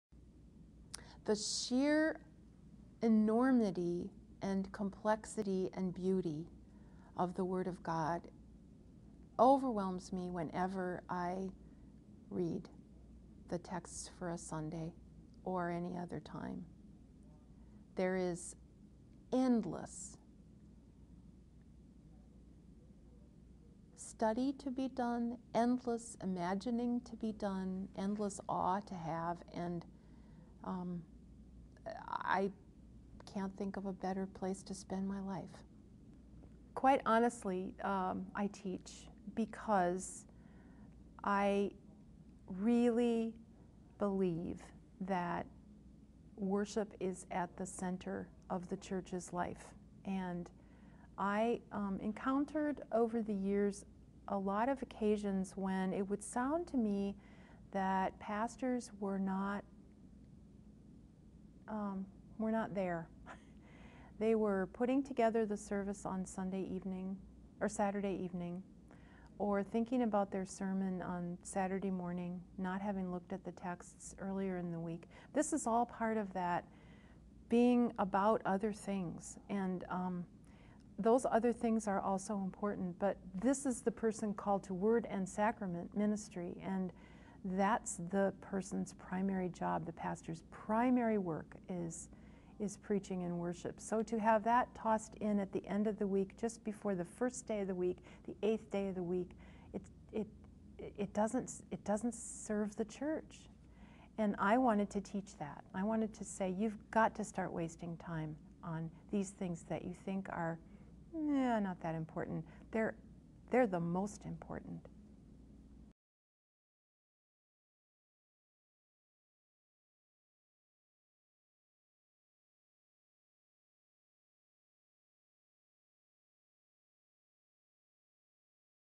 Preaching Moment 132: Melinda Quivik
Quivik talks about seeing worship as the center of the church's life